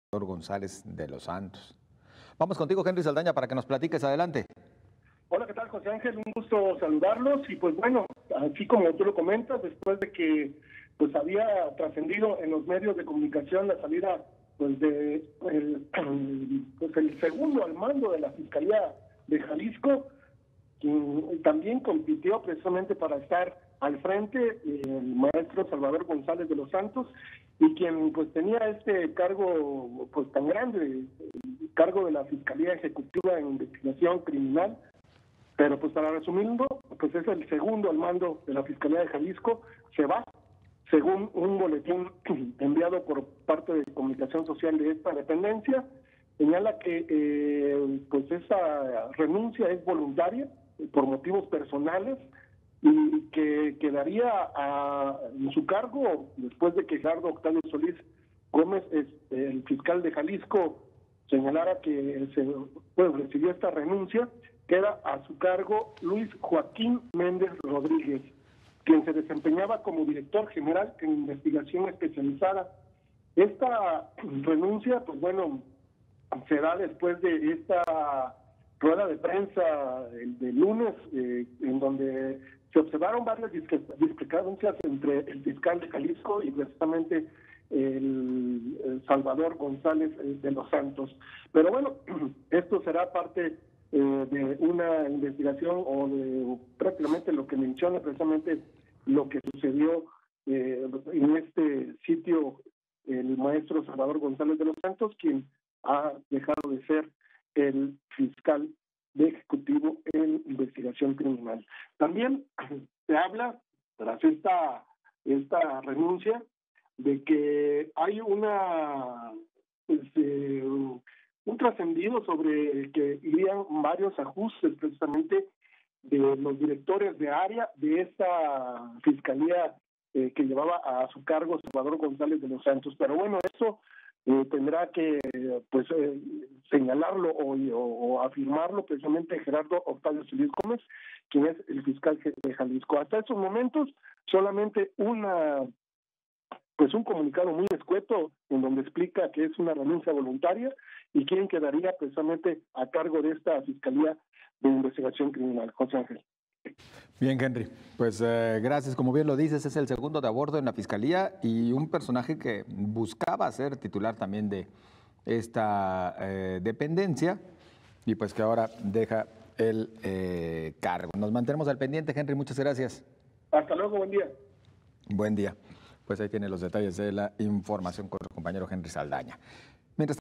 se confirma la salida del fiscal: el titular de la Fiscalía Ejecutiva en Investigación Criminal
se confirma la salida del fiscal: el titular de la Fiscalía Ejecutiva en Investigación Criminal, el Maestro Salvador González de los Santos